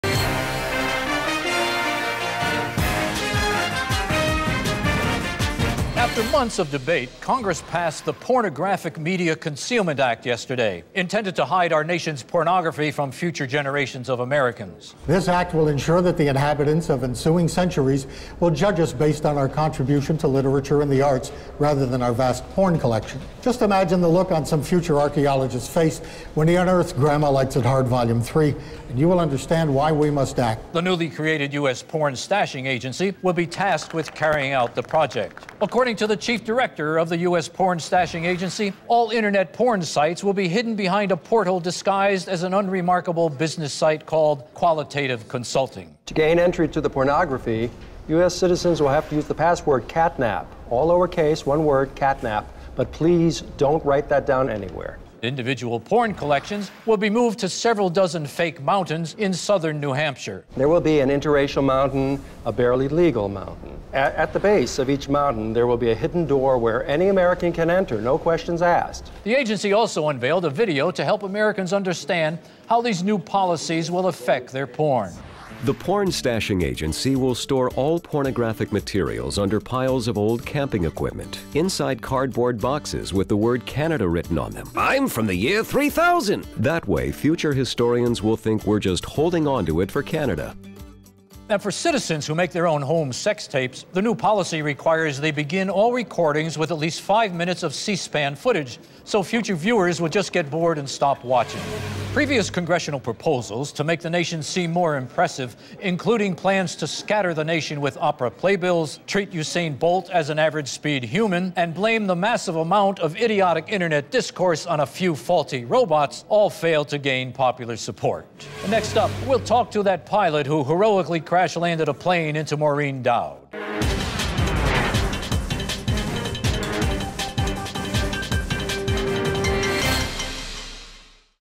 Newsroom : Congress Announces Plan To Hide Nation's Porn From Future Generations
Congress hopes the Pornographic Media Concealment Act will ensure a lasting, respectable legacy for our nation, unmarred by the massive quantities of filthy porn we regularly consume.